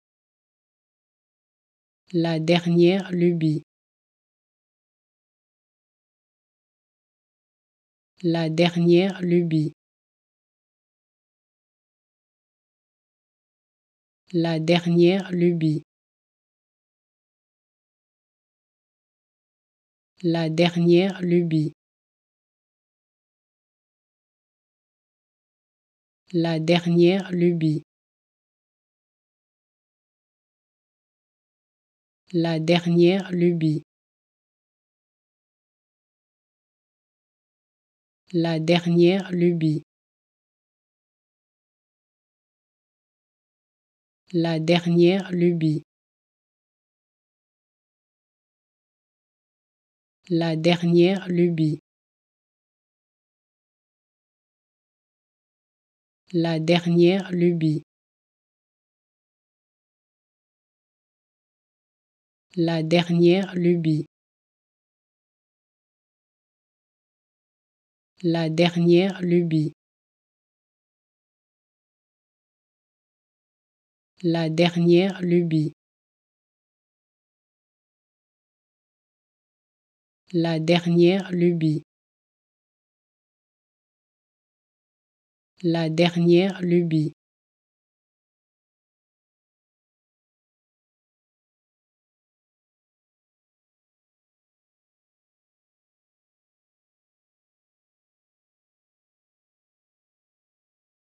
How to Pronounce ''La dernière lubie'' (The latest fad) Correctly in French
Learn how to say and properly pronounce ''La dernière lubie'' in French with this free pronunciation tutorial.